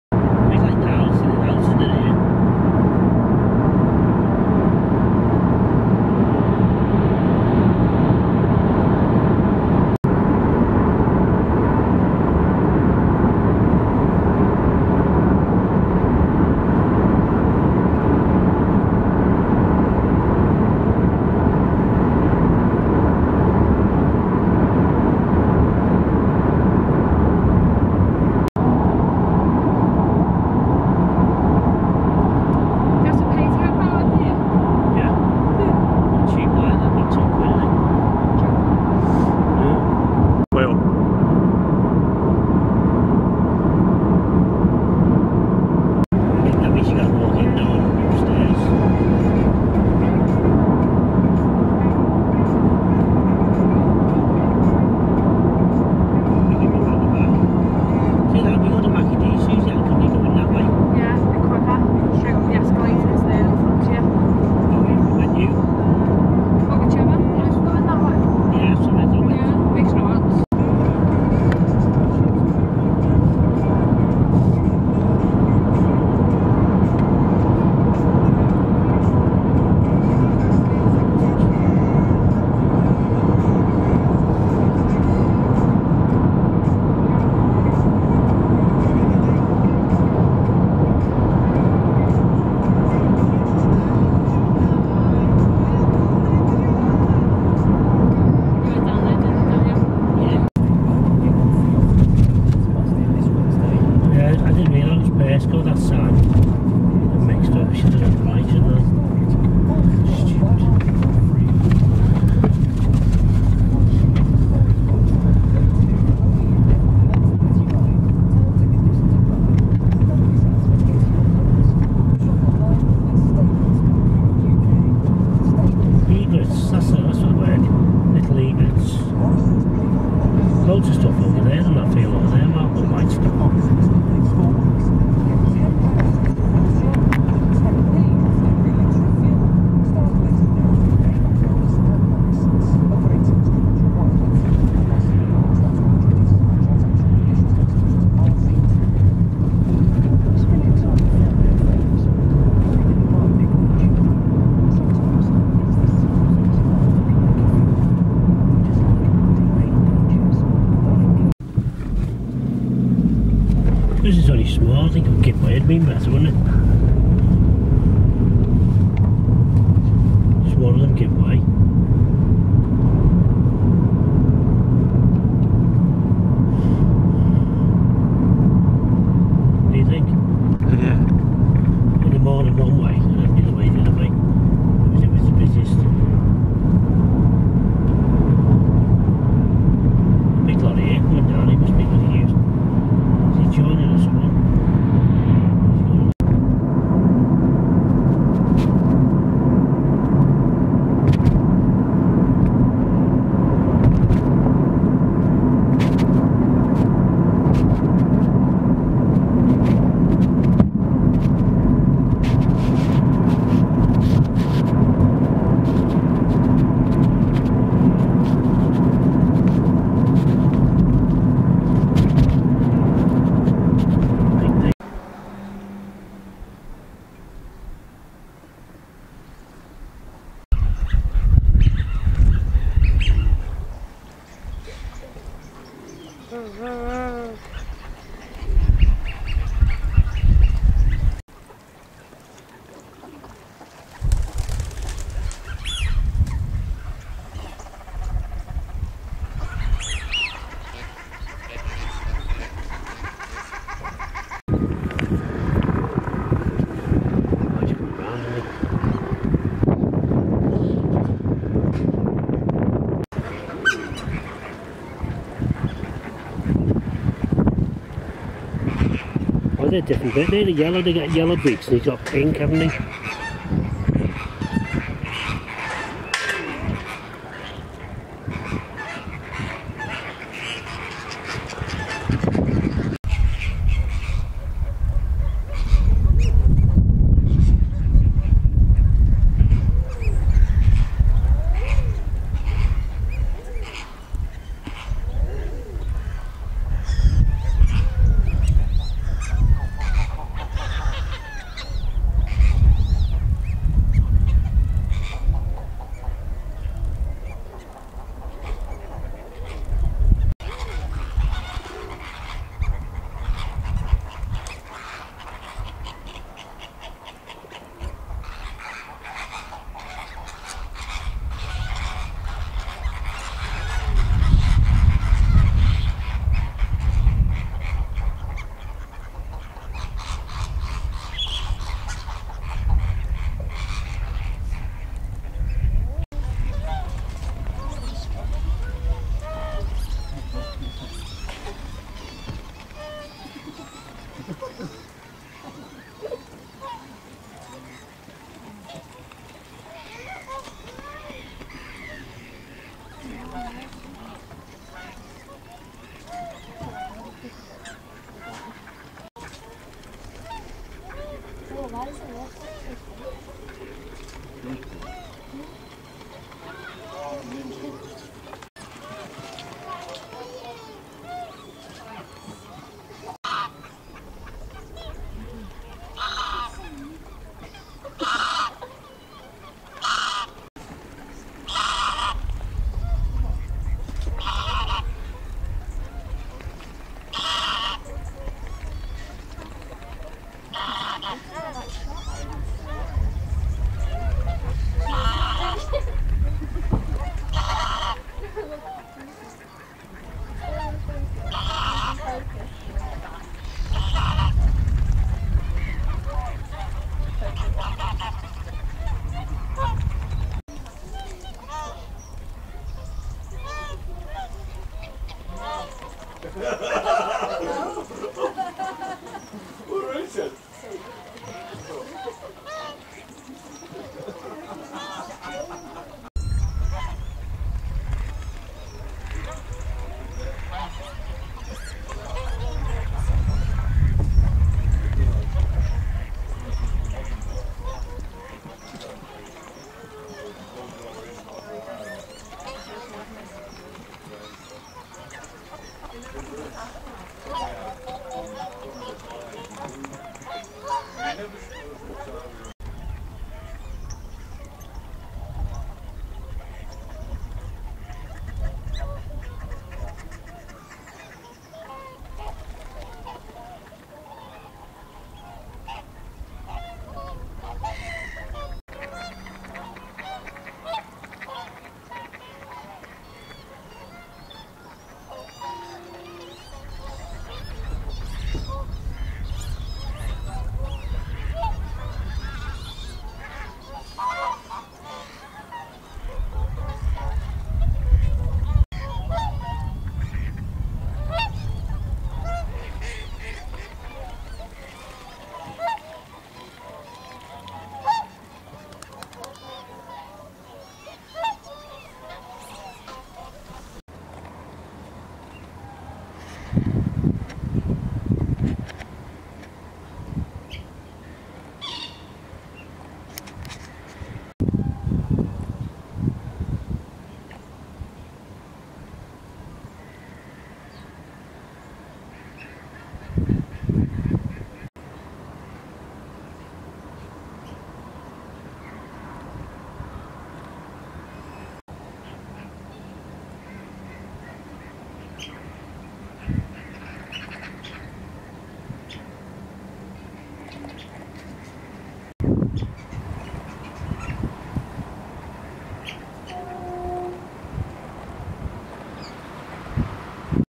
Random drive & loads of birds.
Just a quick mess around. Need to practise my nature photography! It was windy and I didn't have a tripod though...

For those interested, recorded on a Canon EOS 70D with an EF-S 17-55mm f/2.8 and an EF 70-300 L f/4-5/6